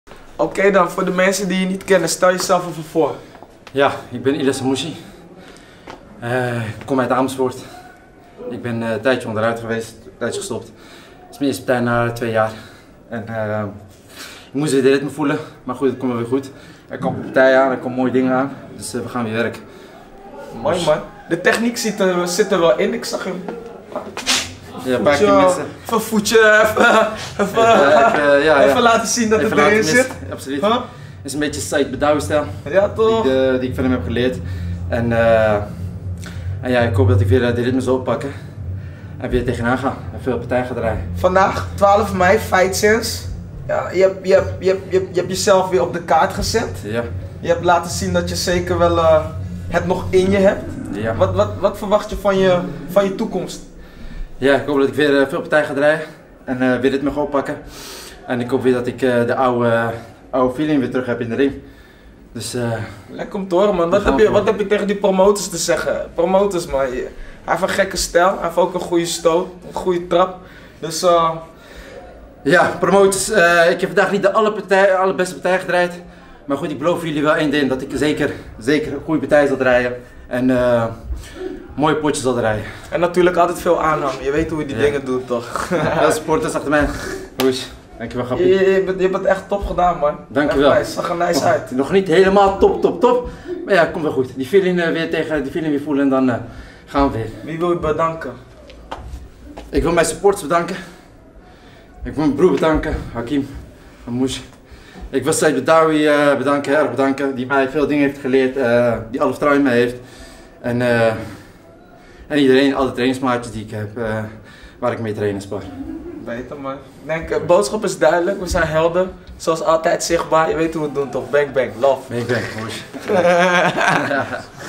Iliass Hammouche Interview - Fightsense 12-05-2018 - Zuiderpark Den Haag
Interview met Iliass Hammouche op het Fightsense presents Enfusion Kickboxing Gala in het Sportcampus Zuiderpark, Den Haag op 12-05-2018.